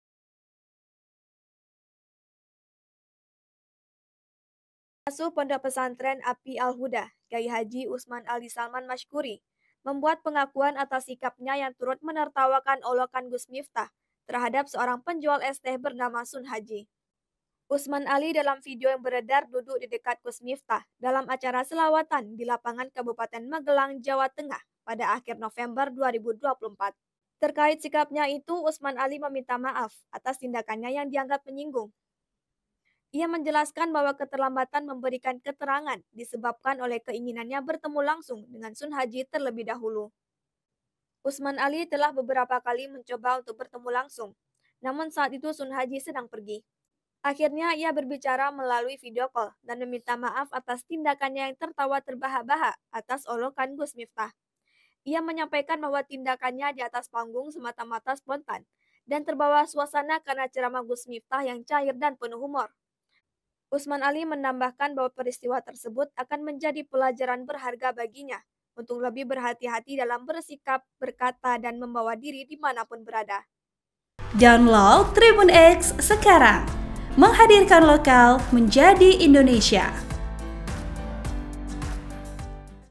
Pengakuan KH Usman Ali Salman Terbahak Saat Gus Miftah Olok Penjual Es Teh: Saya Minta Maaf
TRIBUN-VIDEO.COM - Pengasuh Pondok Pesantren (Ponpes) API Al-Huda, KH Usman Ali Salman Masykuri membuat pengakuan atas sikapnya yang turut menertawakan olokan Gus Miftah terhadap seorang penjual es teh bernama Sunhaji.

Usman Ali dalam video yang beredar duduk di dekat Gus Miftah dalam acara selawatan di Lapangan Kabupaten Magelang, Jawa Tengah pada akhir November 2024.

Terkait sikapnya itu, Usman Ali meminta maaf atas tindakannya yang dianggap menyinggung.

Ia menjelaskan bahwa keterlambatan memberikan keterangan disebabkan oleh keinginannya bertemu langsung dengan Sunhaji terlebih dahulu.

Usman Ali telah beberapa kali mencoba untuk bertemu langsung, namun saat itu Sunhaji sedang pergi.

Akhirnya, ia berbicara melalui video call dan meminta maaf atas tindakannyanya yang tertawa terbahak-bahak atas olokan Gus Miftah.

Ia menyampaikan bahwa tindakannya di atas panggung semata-mata spontan dan terbawa suasana karena ceramah Gus Miftah yang cair dan penuh humor.

Usman Ali menambahkan bahwa peristiwa tersebut akan menjadi pelajaran berharga baginya untuk lebih berhati-hati dalam bersikap, berkata, dan membawa diri di mana pun berada.

Editor Video: Novri Eka Putra
Host: Afifah Zahrah J